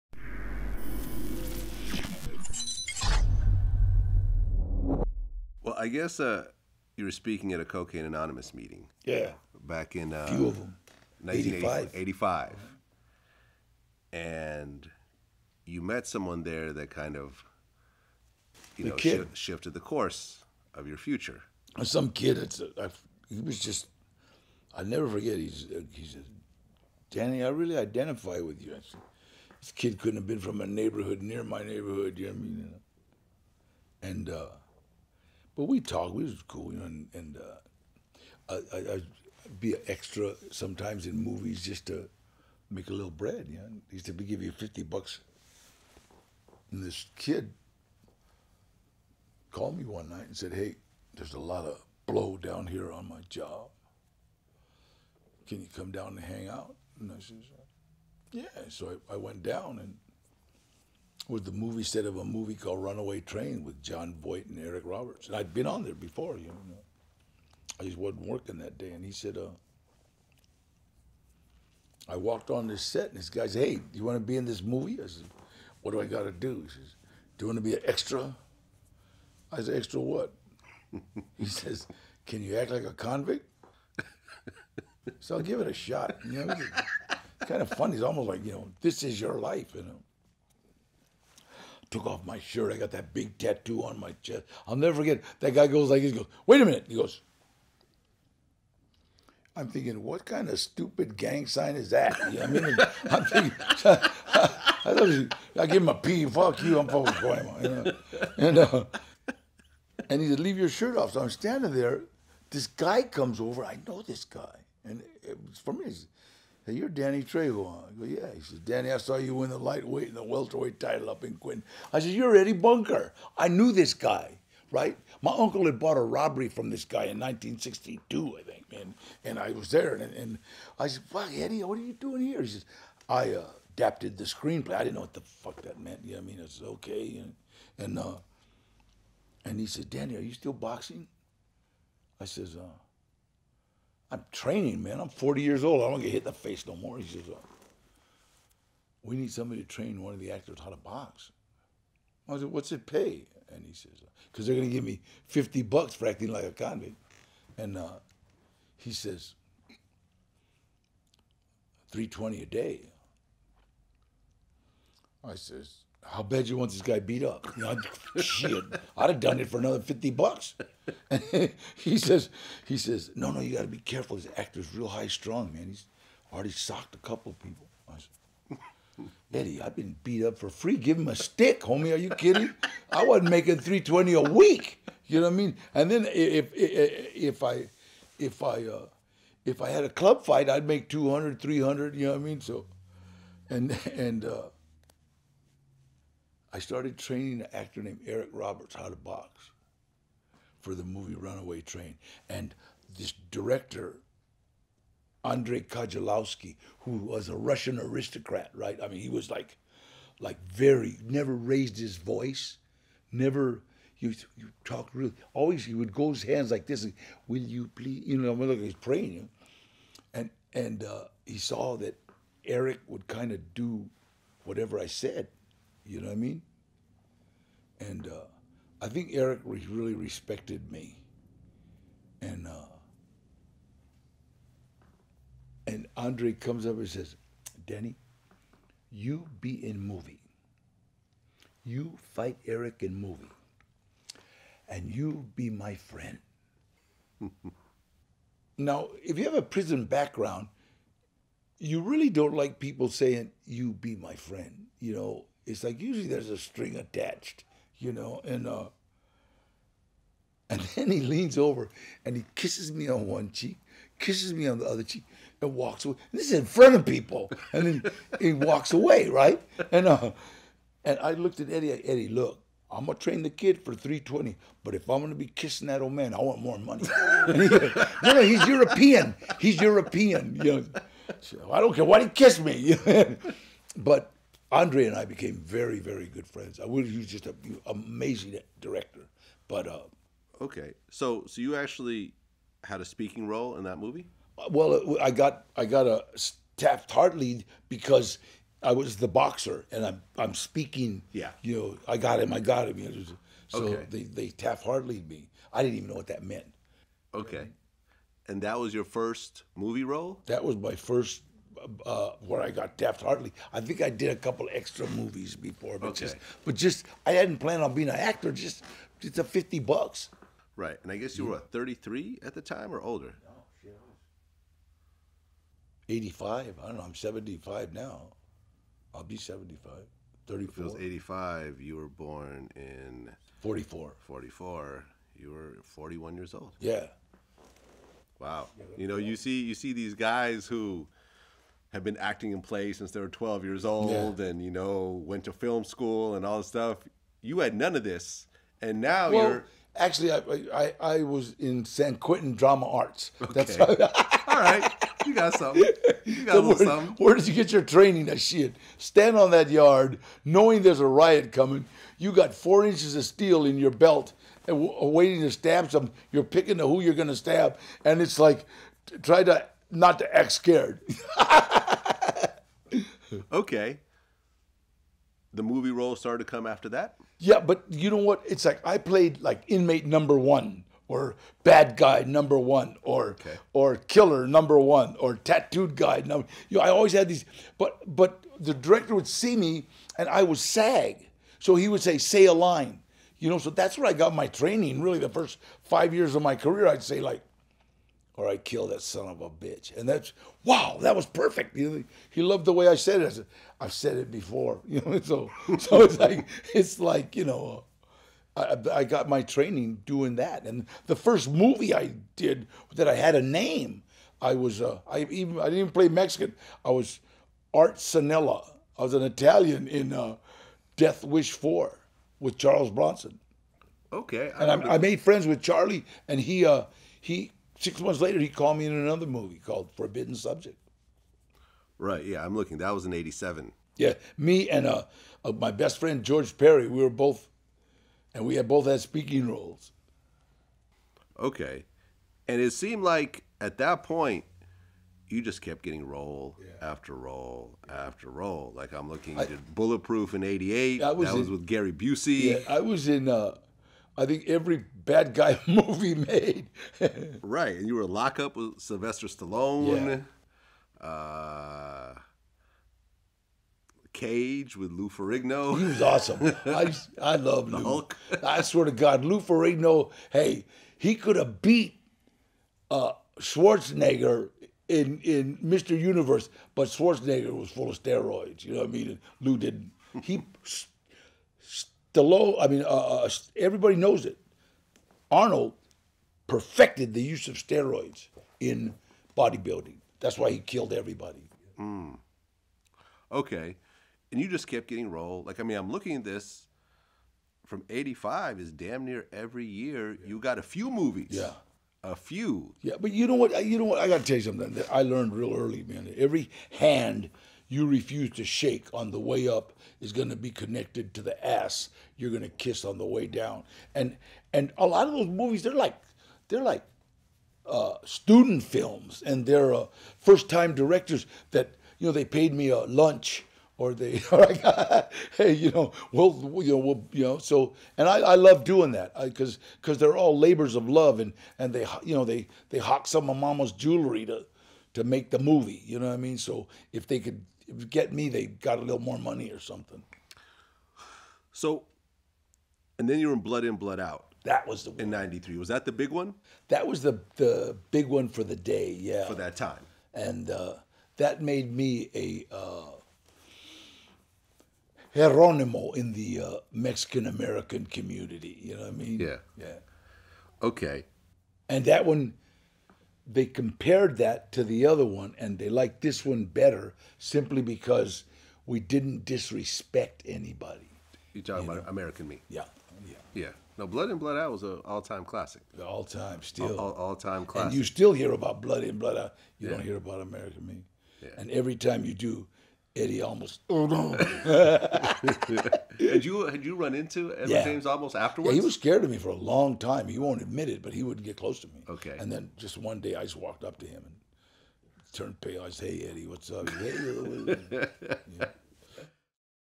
Danny Trejo on Running Into a Terrified Edward James Olmos After 'American Me' (Part 8)
In this clip, Danny Trejo speaks on the events that led up to him landing his first movie role as a boxer in the 1985 film "Runaway Train," which spawned from training actor Eric Roberts for one of the films leading roles alongside Jon Voight, a performance that garnered Roberts a nomination that year for Best Supporting Actor. Trejo notes that he was 41-years-old at that time, as it's mentioned that his path to Hollywood was far different than your standard actor in the industry. All the same, Trejo was soon offered role after role and was featured in multiple films throughout the next decade, one of which was "Blood In Blood Out" that was partially filmed inside of San Quentin.